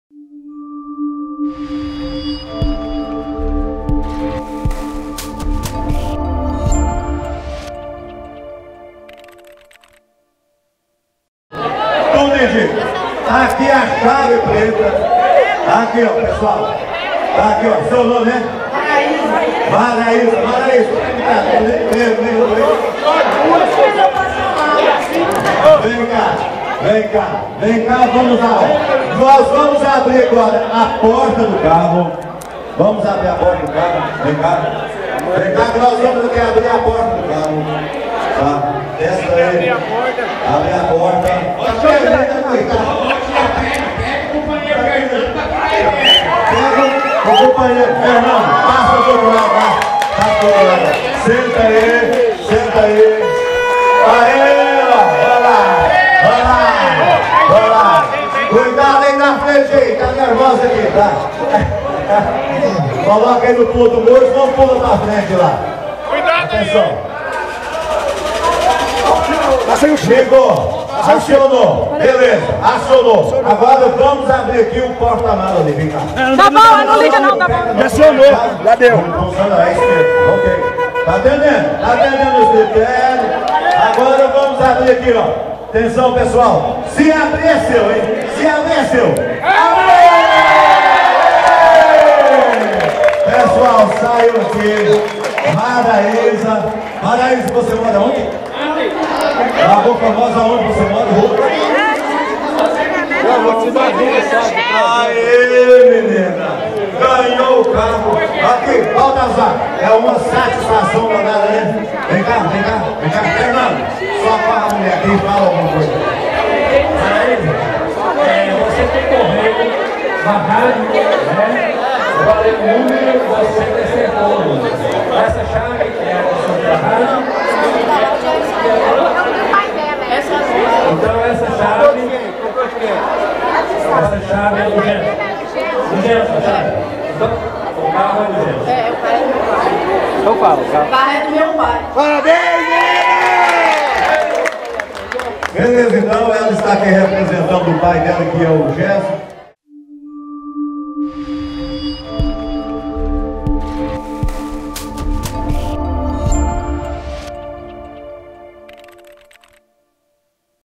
GANHADOR DO CARRO DA PROMOÇÃO CHAVE PREMIADA ROTARY CLUBE
Esta semana foi realmente muito movimentada, para o Rotary Clube de Lagoa Formosa, na terça feira aconteceu uma importante reunião com o prefeito municipal e os vereadores de Lagoa Formosa na sede clube de serviços